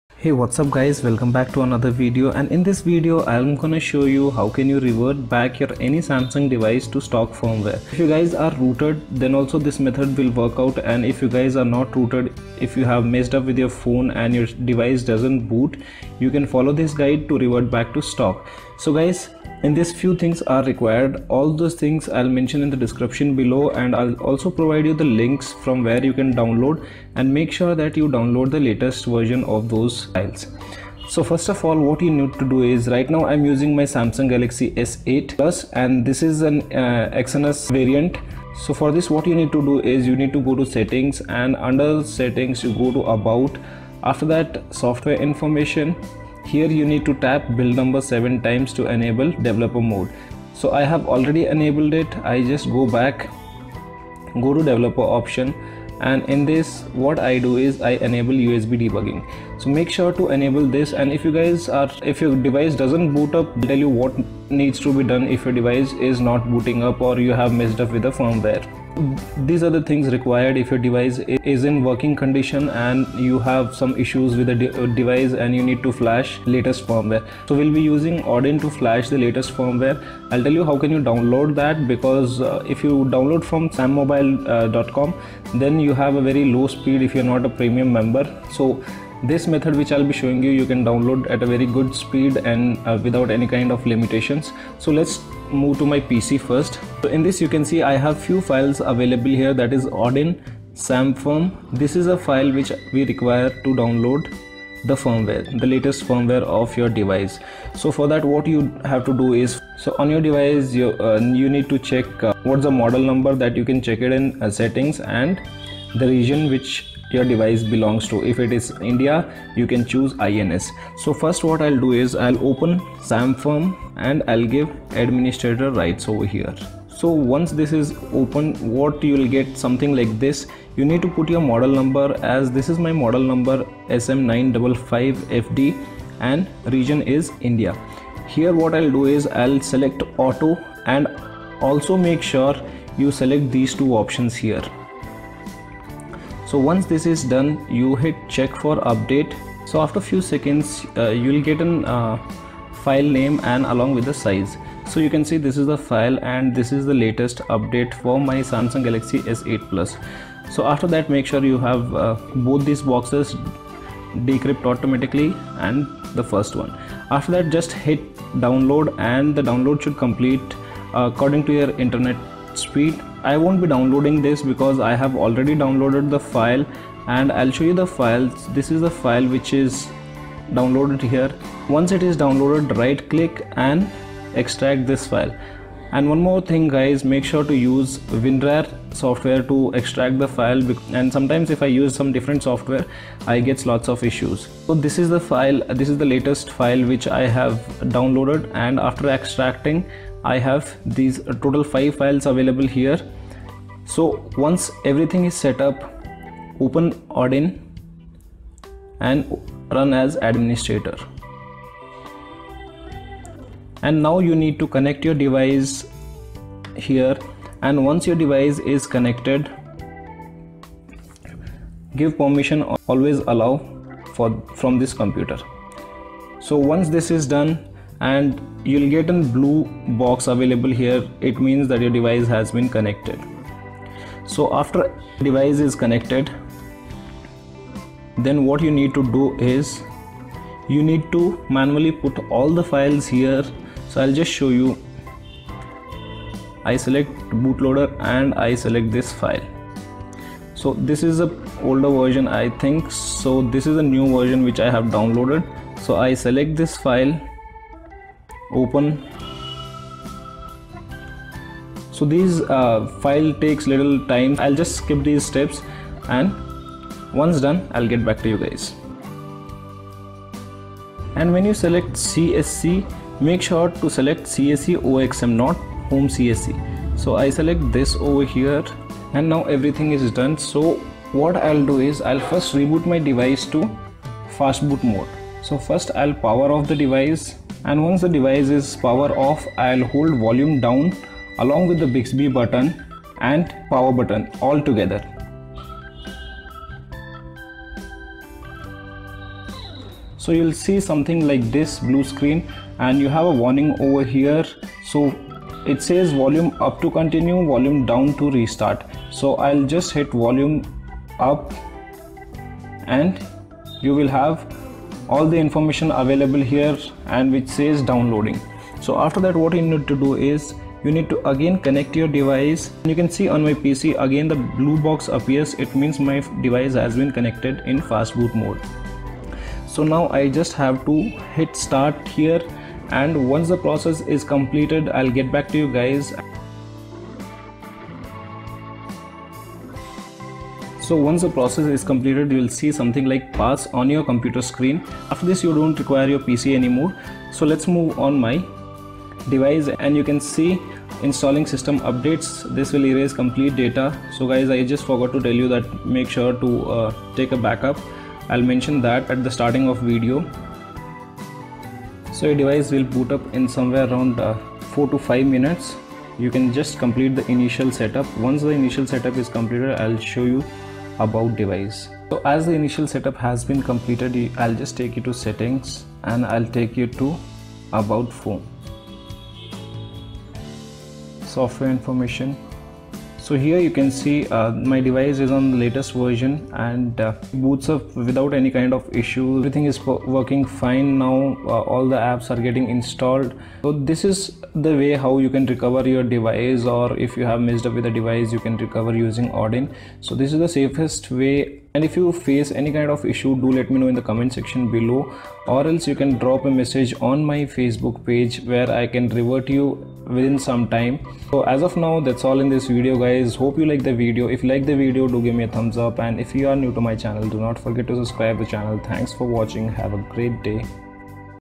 | Samsuns S8 & S8+ | How to Revert any samsung device to Stock | ROOTED & NON ROOTED |
This method can be used to revert any samsung device to stock if you guys face any kind of issues and this method can also be used to update to latest software.

***IMPORTANT***
TAKE BACKUP this will erase complete data.

Rest follow this guide